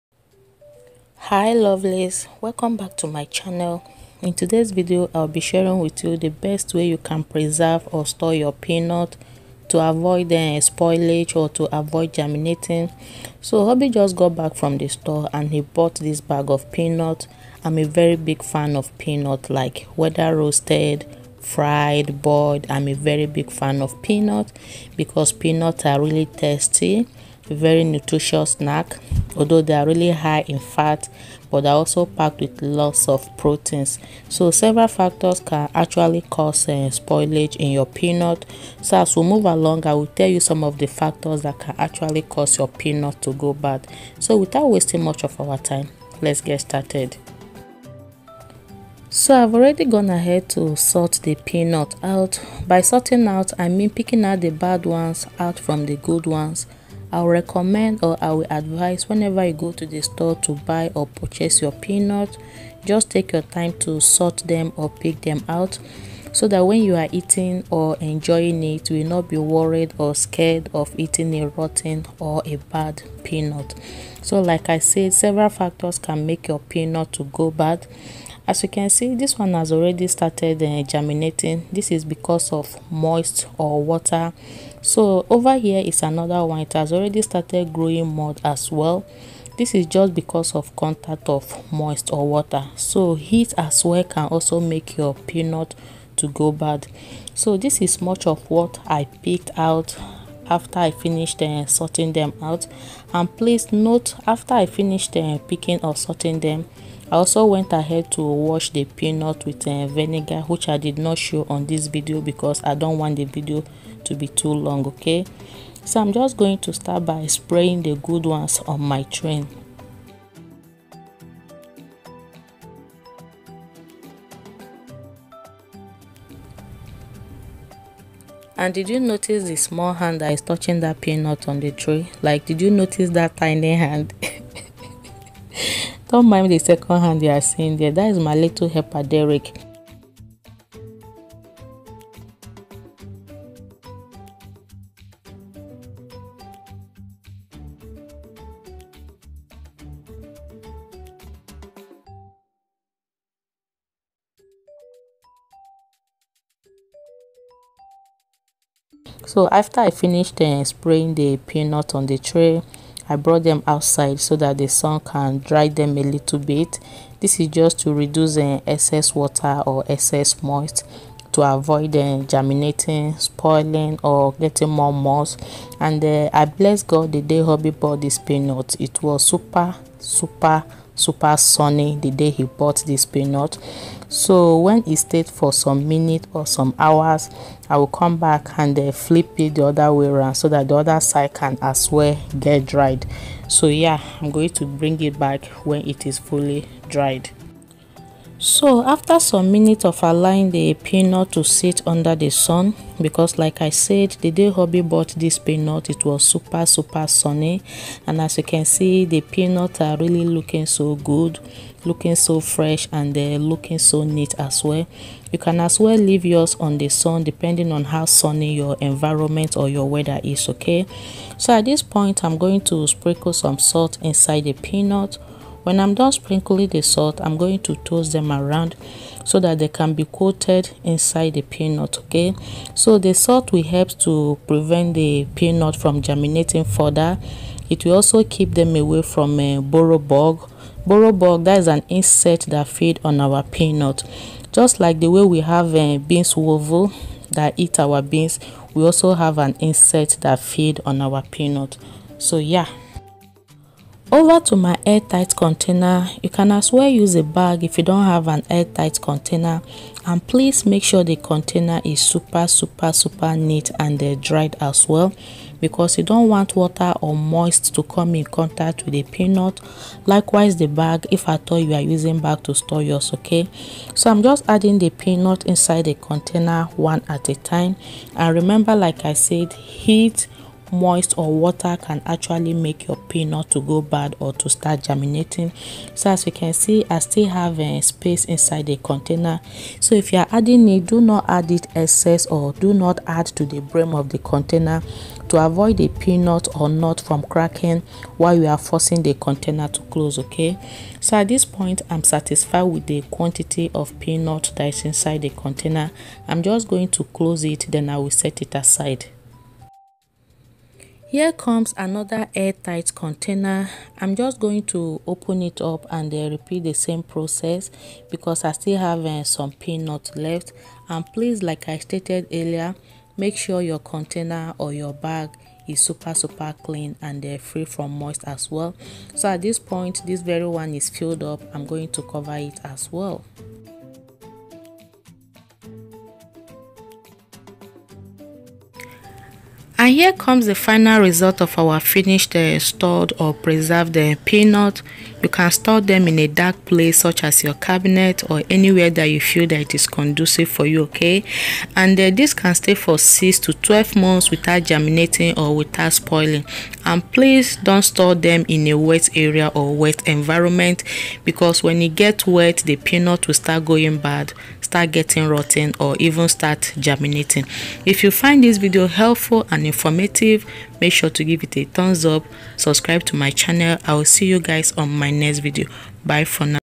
RAW PEANUT IN THE SHELL | EASY WAY TO STORE & PRESERVE RAW PEANUT FOR A LONG TIME
Hi lovelies today's video I shared an easy way to store or preserve your raw peanuts from spoilage or from getting germinated.

This method is really effective, very easy to follow. ..you can store them as long as 12 months without getting spoilt. ..but please avoid putting them close to humid  (wet)to avoid germination.

Burrower bugs are insects that always feed on peanuts especially when they are not properly stored, the bugs are tiny that if you don't look closely you might not see them.

Peanut are a good snack, which is eaten by many people ,,,you can boil, roast,or fry it according to how you want to enjoy it.

So if you are staying where peanuts are seasonal you can as well get them as much as you can store, preserve them in order not to run out of shortage.

Peanuts are really good although it contains fats but also packed with lots of proteins.

Snap Chat:Faith Sunny 29